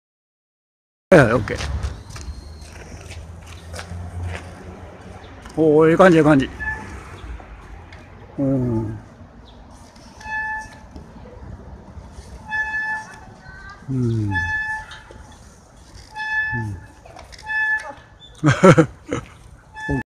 数土くんの大車輪
城南公園で大車輪をしていると、中学生の仲間ができました。大車輪の輪をもっと大きく広げていきたいですね。大車輪に関心のある人、城南公園に集合！